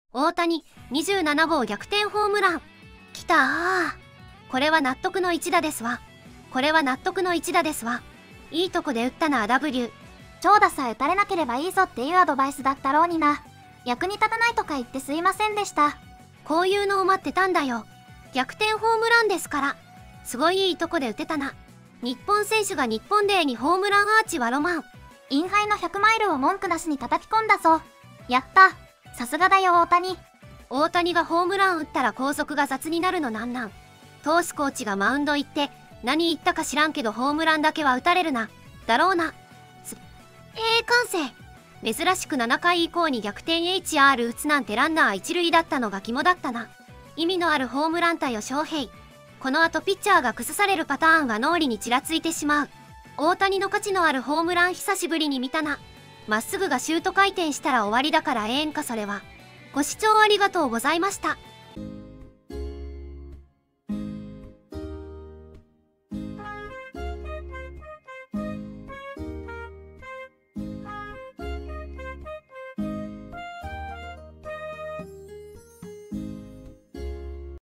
【メジャー500打点達成！！！】大谷 27号逆転ホームラン！【プロ野球反応集】【1分動画】
引用元　【LAD】大谷翔平応援スレ part2273【TWP】

LAMP BGM